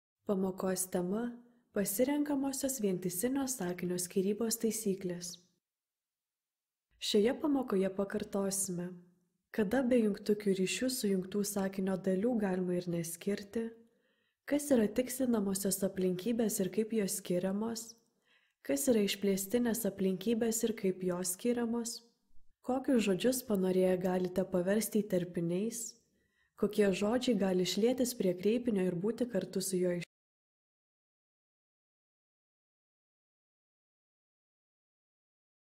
Pasirenkamosios vientisinio sakinio skyrybos taisyklės - Mokslinčius.lt - Tavo mokytojas telefone !
Šios pamokos metu pasikartosime, kada bejungtukiu ryšiu sujungtų sakinio dalių galima ir neskirti, kas yra tikslinamosios aplinkybės ir kaip jos skiriamos, kas yra išplėstinės aplinkybės ir kaip jos skiriamos, kokius žodžius panorėję galite paversti įterpiniais, kokie žodžiai gali šlietis prie kreipinio ir būti kartu su juo išskiriami bei kas gali būti rašoma vietoje praleistos tarinio jungties ar kitos sakinio dalies.